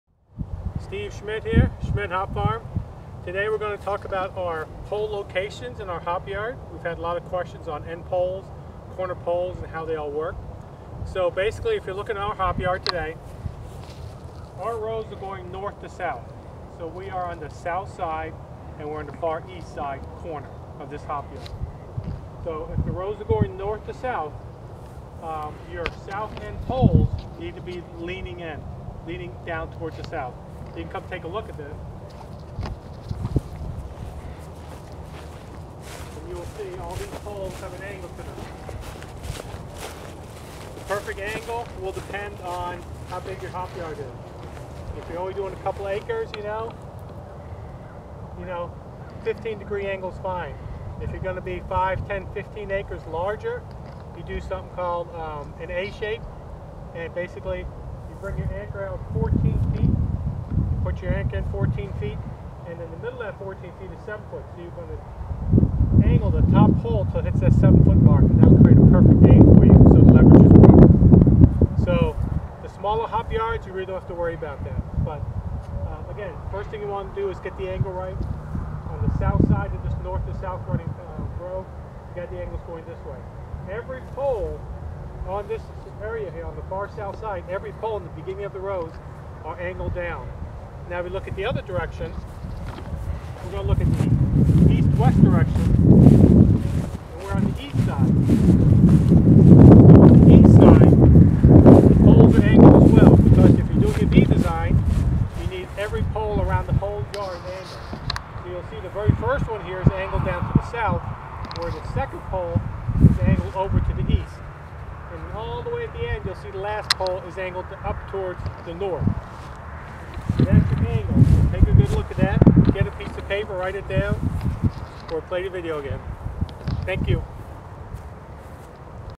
Pole Direction of Lean
Show which direction poles have to lean for a hop yard.